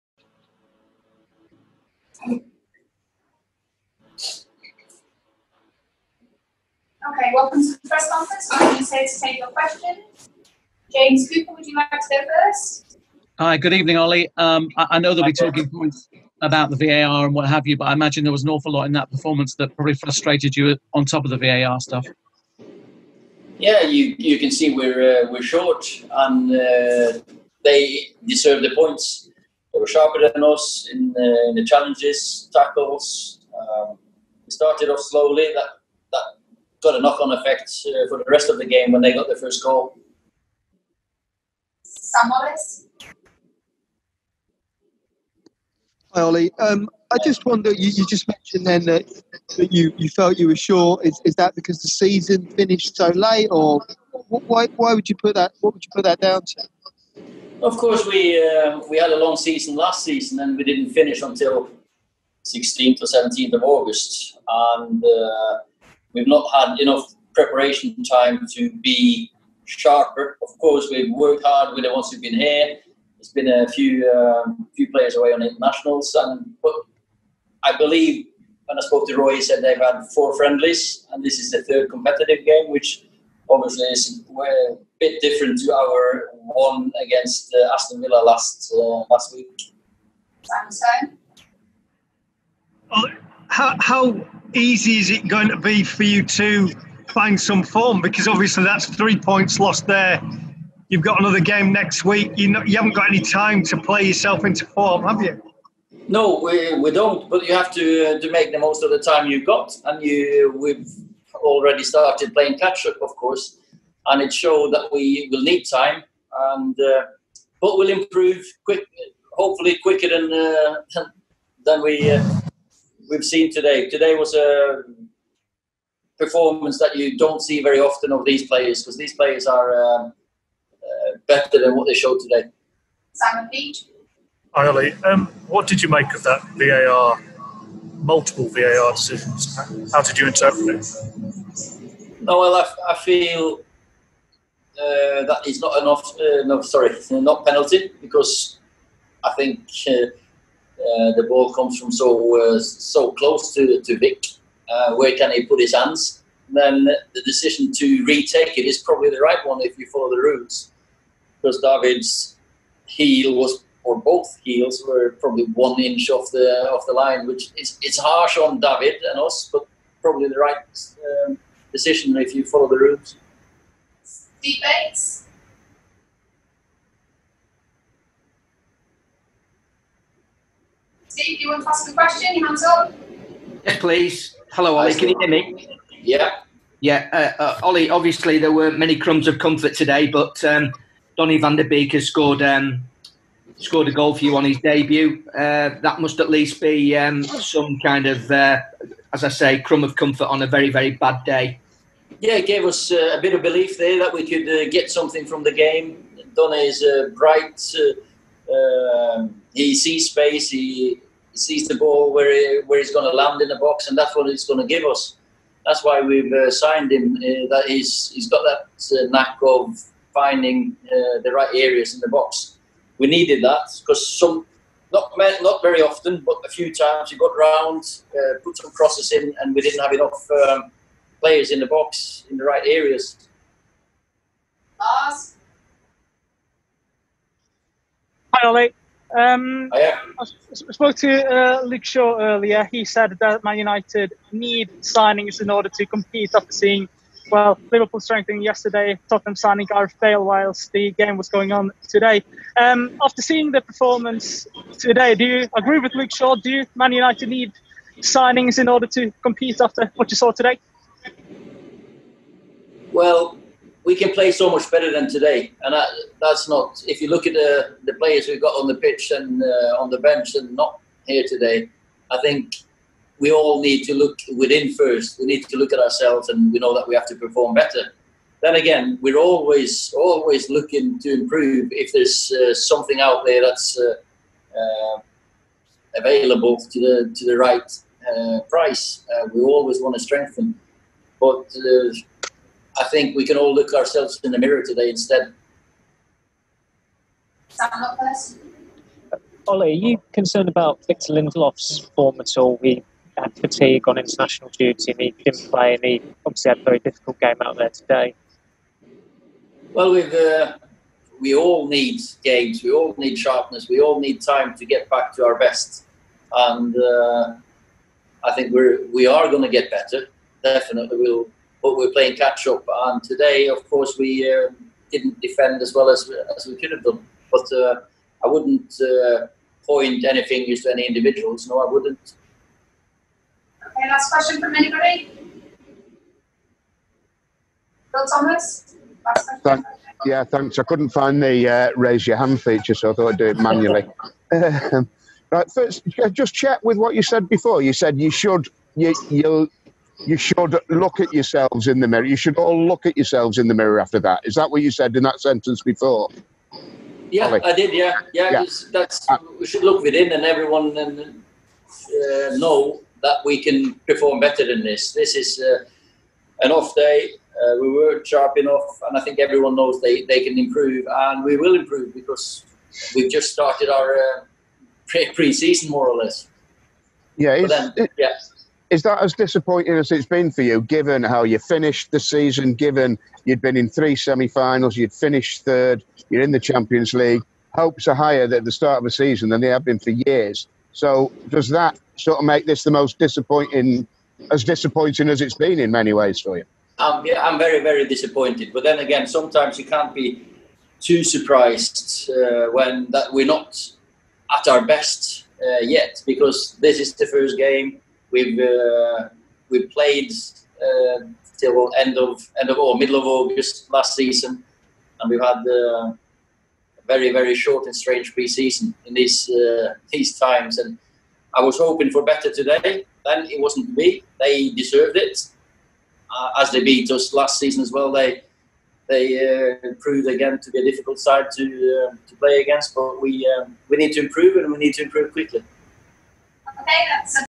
Man Utd 1-3 Crystal Palace - Ole Gunnar Solskjaer - Post Match Press Conference
Man United manager Ole Gunnar Solskjaer speaks after his side's 1-3 defeat at home against Crystal Palace in the Premier League.
Hear from Ole Gunnar Solskjaer in his post match press conference after the Reds suffered defeat to Crystal Palace at Old Trafford.